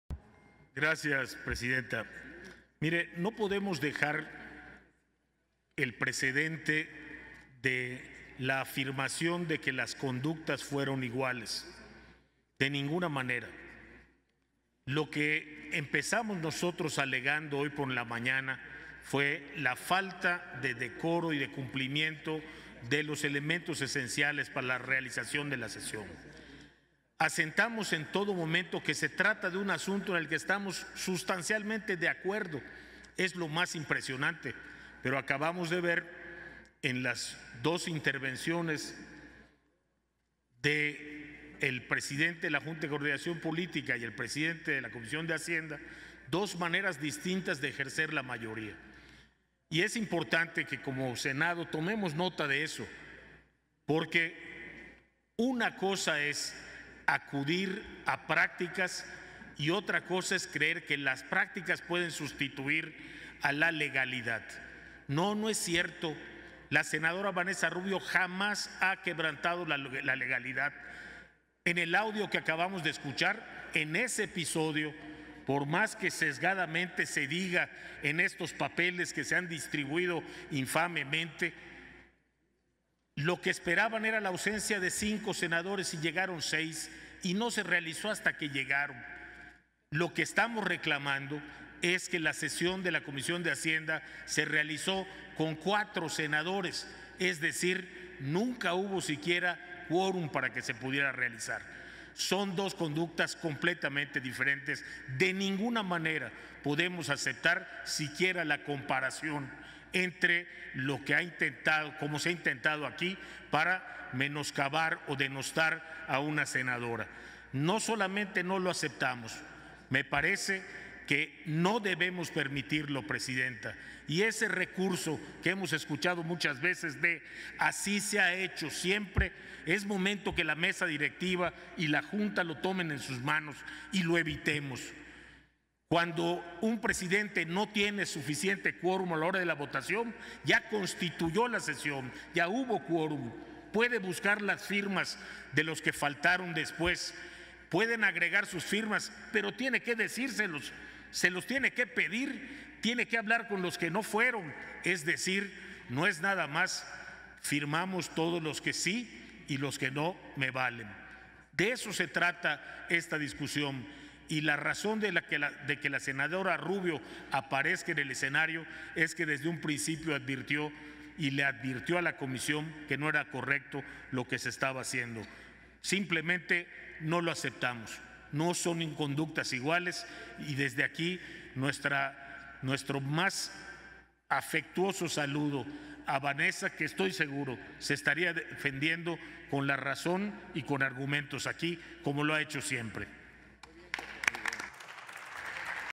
Tercera intervención desde su escaño Sen. Jorge Carlos Ramírez Marín - 19 de Marzo 2020
Tercera intervención desde su escaño, del senador por el Partido Revolucionario Institucional, Jorge Carlos Ramírez Marín. Sesión Ordinaria.

20 de Marzo 2020.